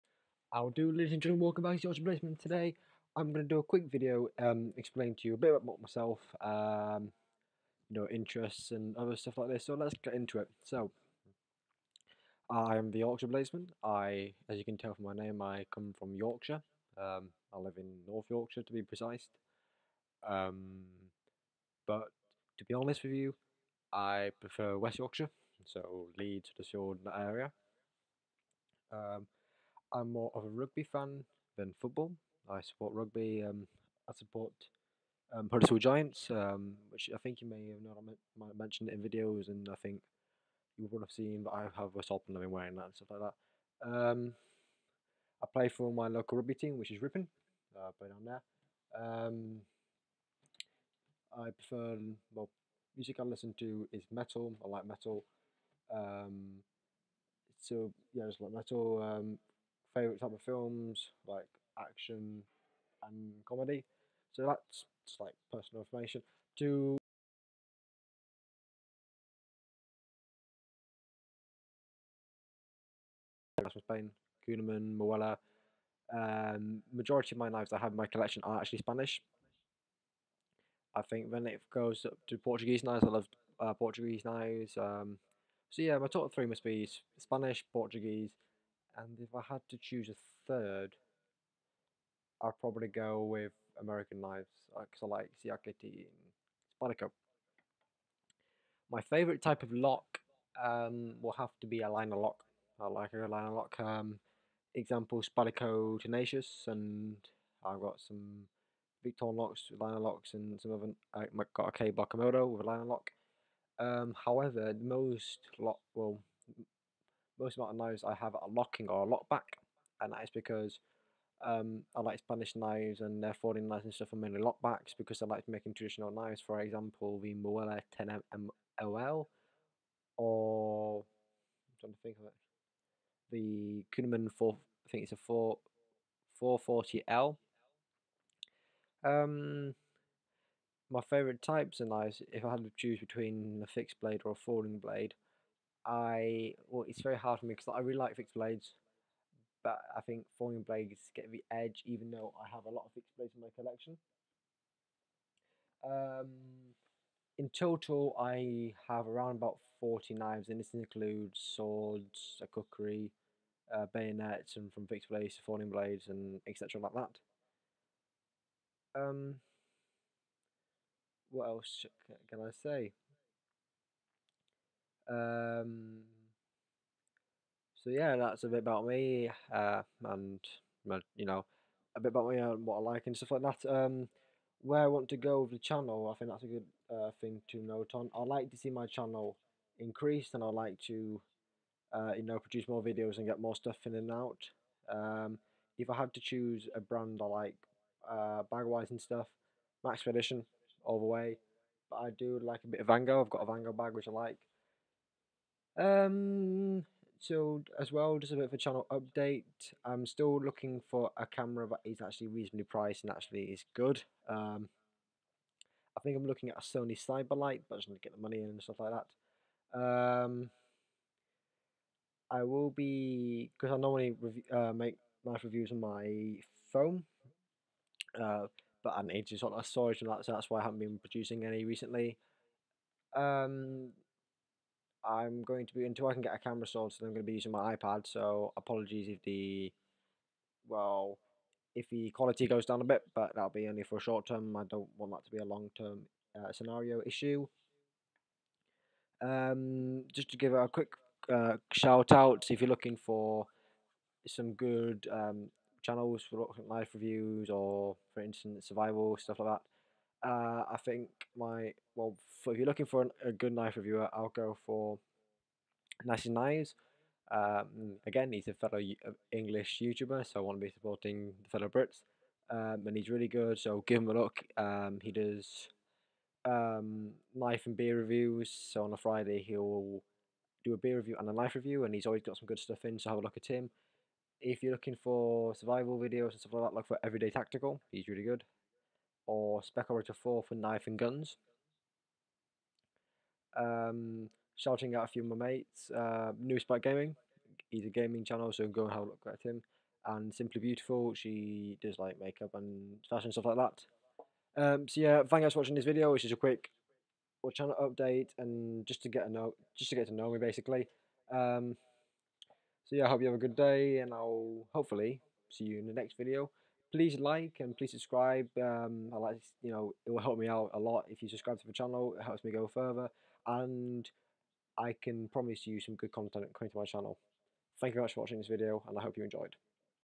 Getting to know The Yorkshire Bladesman
Here is a quick video expelling some of my interprets and preferences of knives to you.